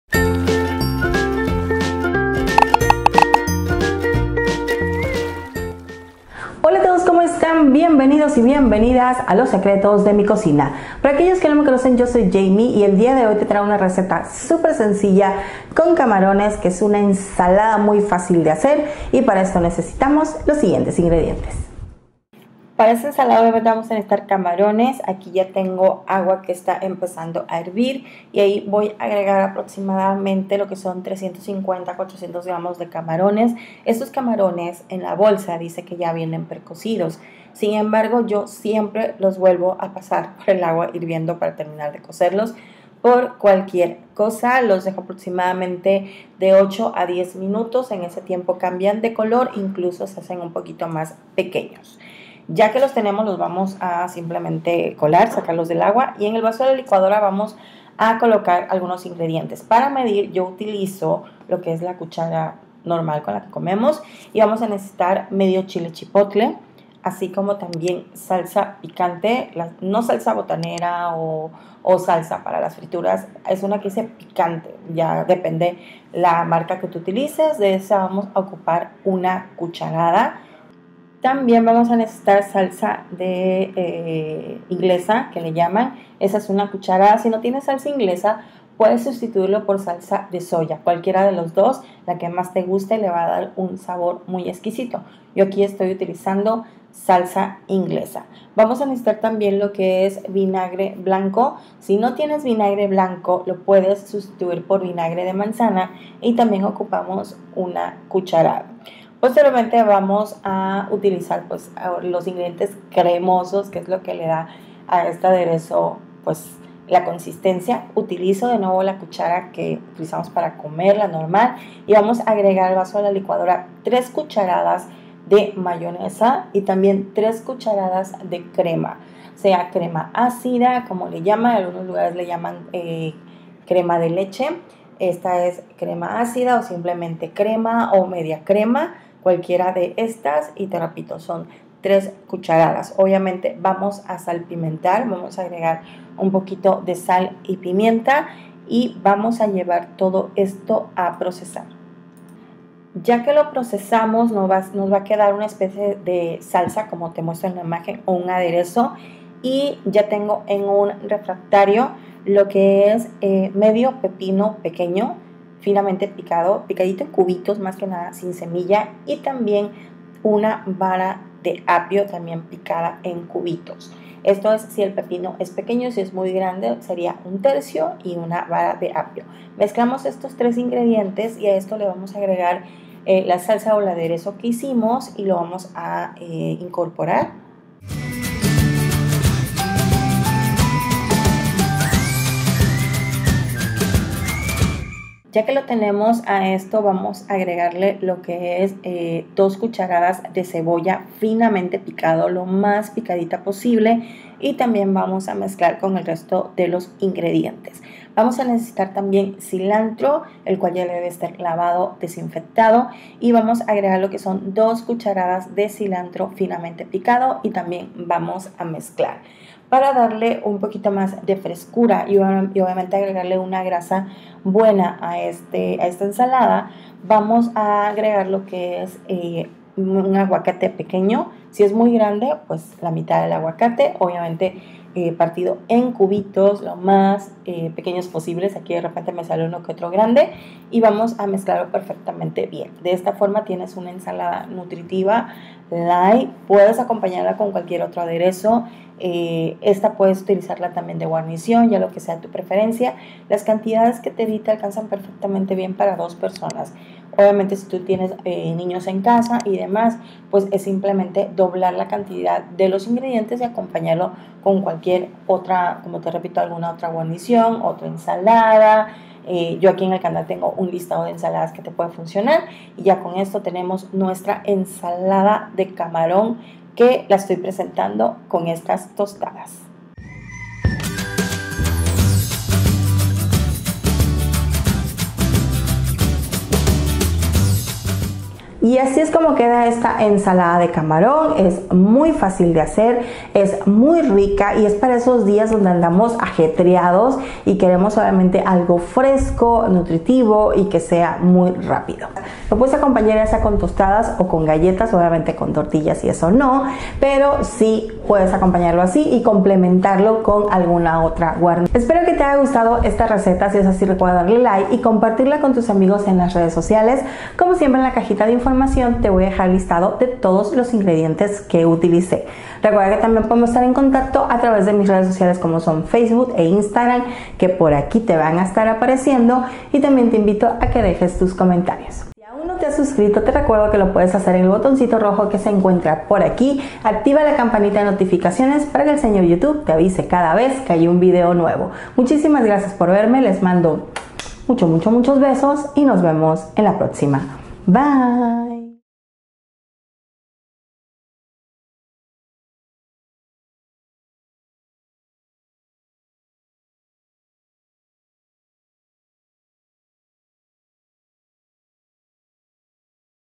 ENSALADA DE CAMARON FACIL🍡LOS SECRETOS DE MI COCINA🍡
Si buscas algo fresco, rapido y ligth esta receta es para ti, ensalada de camaron facil los secretos de mi cocina.
🍴Ingredientes:
400 grs de camaron
1 vara de apio
1/2 pepino chico
2 cucharadas de cebolla
2 cucharadas de cilantro
1 aguacate mediano
para el aderezo
1/2 chile chipotle
1 cucharada de salsa picante
1 cucharada de salsa inglesa
1 cucharada de vinagre blanco
3 cucharadas de mayonesa
3 cucharadas de crema àcida

Musica Libreria de Youtube